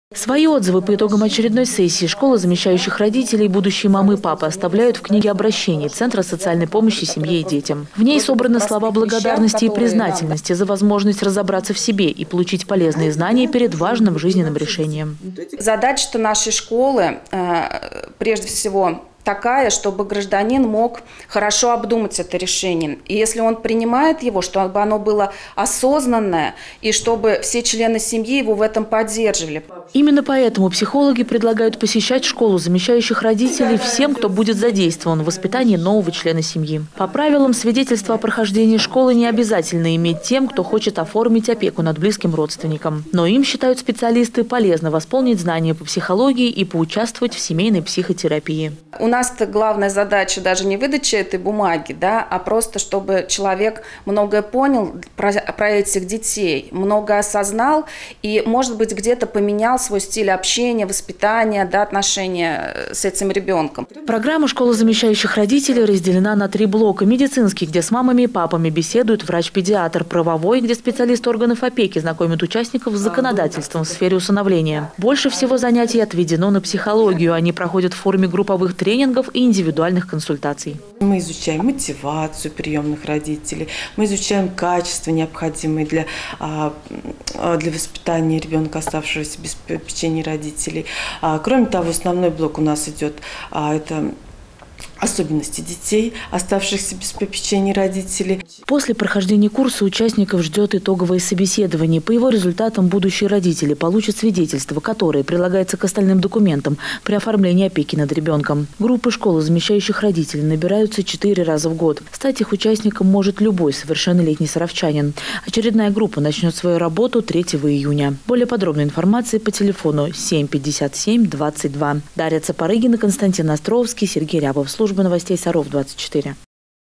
Школа замещающих родителей
Центр социальной помощи семье и детям проводит набор в летнюю группу «Школы замещающих родителей». Занятия дают возможность будущим усыновителям и опекунам получить полноценную правовую и психологическую подготовку к ответственному шагу.